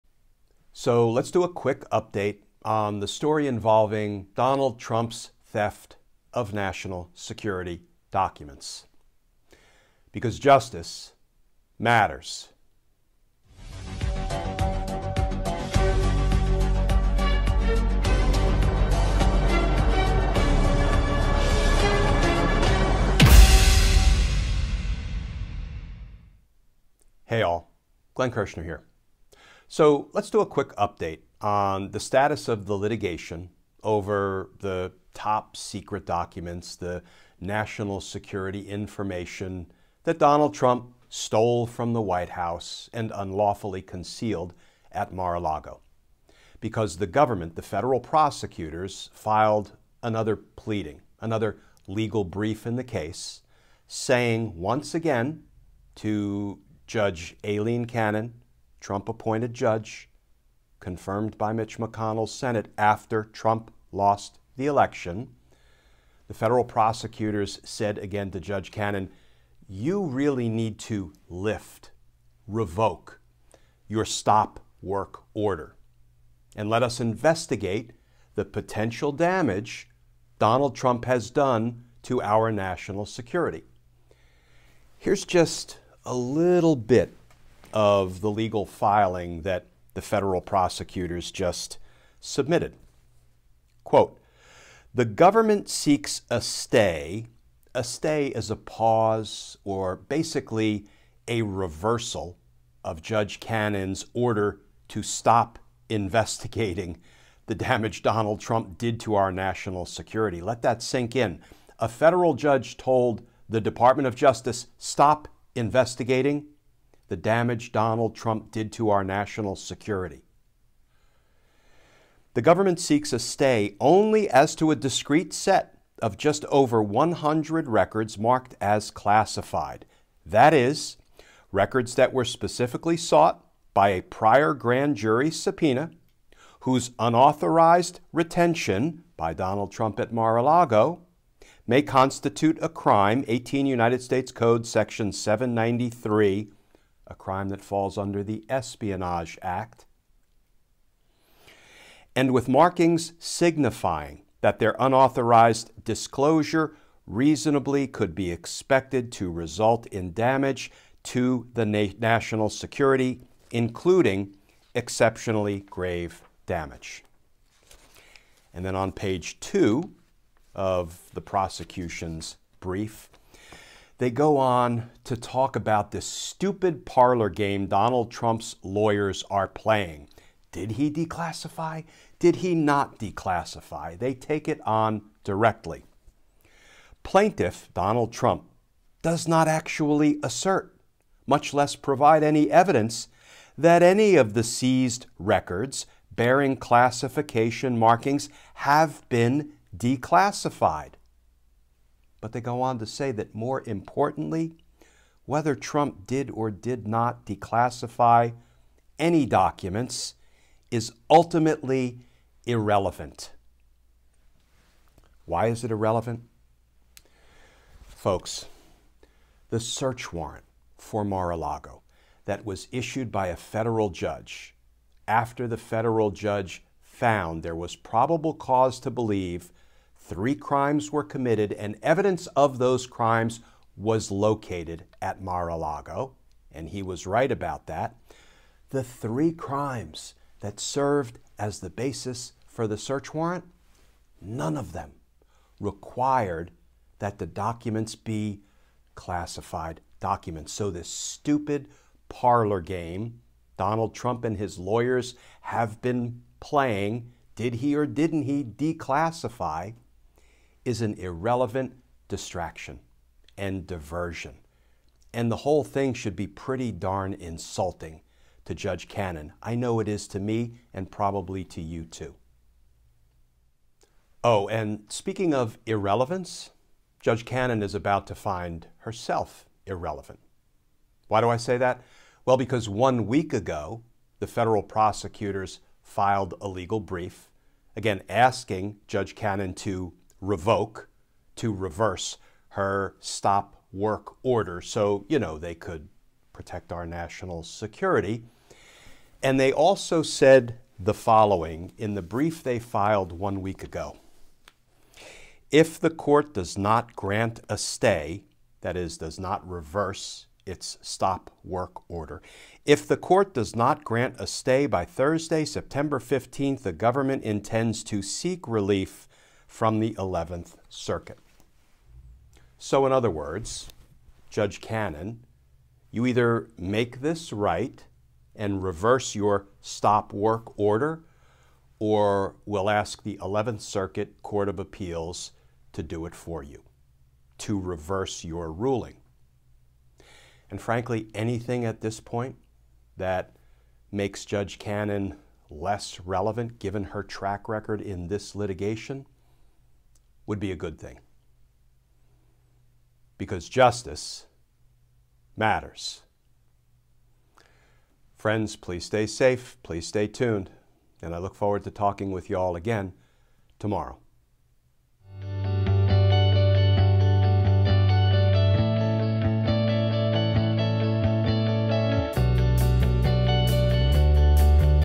DOJ exposes Trump's declassification BS, tells Judge Cannon they'll seek appellate court reversal
Department of Justice federal prosecutors filed another legal brief in the litigation involving Donald Trump's theft and unlawful retention of national defense information. In the new pleading, the prosecutors expose Trump's absurd parlor game of "did-he-or-didn't-he-decalssify-documents" as "irrelevant." Prosecutors also tell the presiding judge, Aileen Cannon, that if she doesn't reverse her dangerous stop work order - telling prosecutors they are prohibited from investigating the damage Trump may have done to our national security - then on Thursday, September 15, they will ask the 11th Circuit Court of Appeals to reverse her ruling and allow the criminal investigation to continue. Here's what prosecutors say in this new court filing.